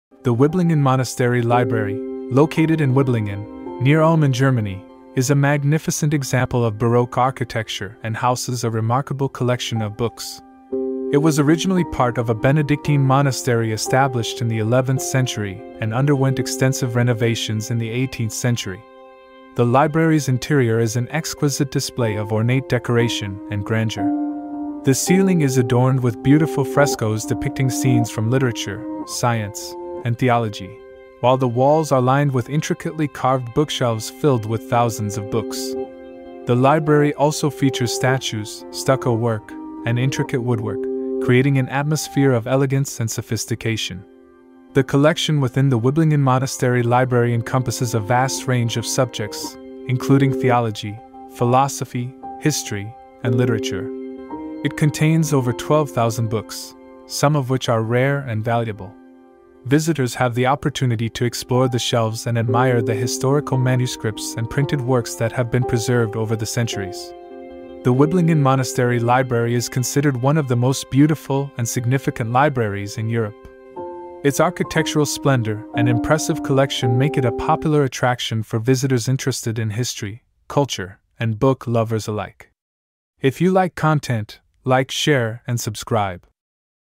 Wiblingen Monastery Library Room 📚, Germany 🇩🇪
The Wiblingen Monastery Library, located in Wiblingen, near Ulm in Germany, is a magnificent example of Baroque architecture and houses a remarkable collection of books. It was originally part of a Benedictine monastery established in the 11th century and underwent extensive renovations in the 18th century.

The library's interior is an exquisite display of ornate decoration and grandeur. The ceiling is adorned with beautiful frescoes depicting scenes from literature, science, and theology, while the walls are lined with intricately carved bookshelves filled with thousands of books. The library also features statues, stucco work, and intricate woodwork, creating an atmosphere of elegance and sophistication.

The collection within the Wiblingen Monastery Library encompasses a vast range of subjects, including theology, philosophy, history, and literature. It contains over 12,000 books, some of which are rare and valuable. Visitors have the opportunity to explore the shelves and admire the historical manuscripts and printed works that have been preserved over the centuries.

The Wiblingen Monastery Library is considered one of the most beautiful and significant libraries in Europe. Its architectural splendor and impressive collection make it a popular attraction for visitors interested in history, culture, and book lovers